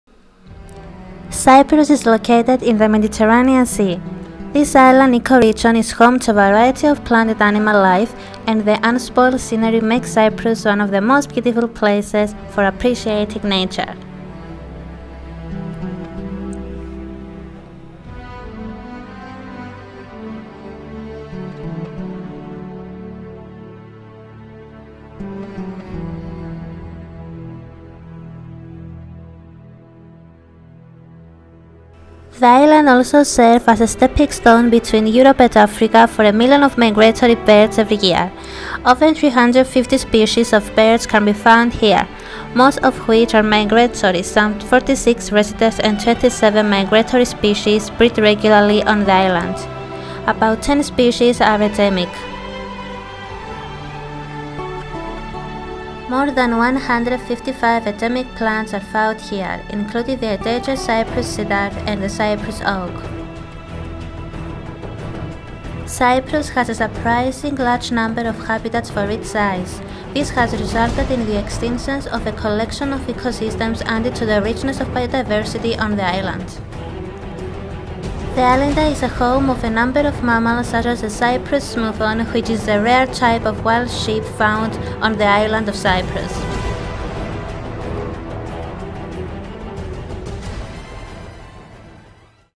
Wildlife and Nature of Cyprus - Science Communication - University of Derby
This is a coursework for the module Science Communication.

It is a short video about the wildlife and nature of Cyprus, illustrates some of the species which can be found on this island as well as some of its habitas.

References:

Nature of Cyprus (2014) Nature of Cyprus. [Online]. (Accessed: 22/2/2014)

OurIslandSeries. ( 2013, April 9) OurIsland cyprus nature & wildlife film Series (presentation). [Video File]. (Accessed: 22/2/2014)

OurIslandSeries. ( 2013, April 9) OurIsland cyprus nature & wildlife film Series Underwater stock footage #002. [Video File]. (Accessed: 22/2/2014)

OurIslandSeries. ( 2013, April 9) OurIsland cyprus nature & wildlife film Series - stock footage #001. [Video File]. (Accessed: 22/2/2014)